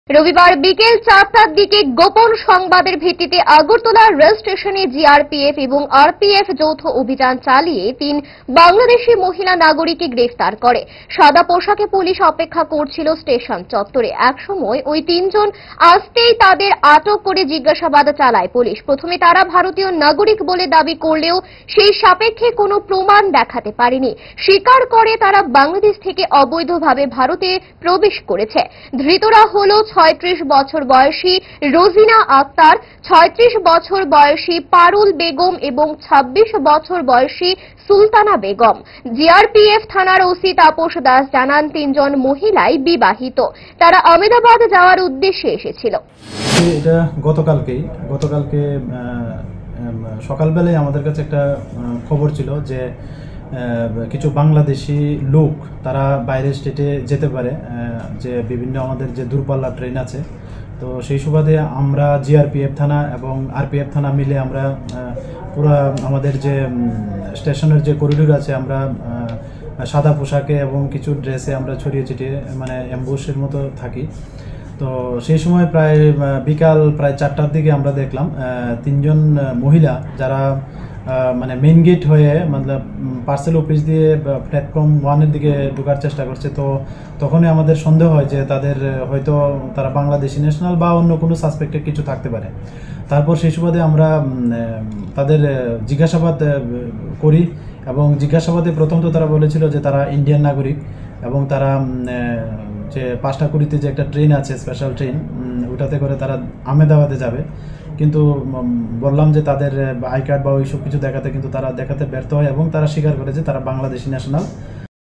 ফের ত্রিপুরায় গ্রেপ্তার অবৈধ ৩ বাংলাদেশী নাগরিক।
ফের ত্রিপুরায় গ্রেপ্তার অবৈধ ৩ বাংলাদেশী নাগরিক। ধৃতরা প্রত্যেক-ই বিবাহিত মহিলা। আগরতলা থেকে রেলে করে আমেদাবাদ যাওয়ার সময় জিআরপিএফ পুলিশের হাতে আটক হয় তারা।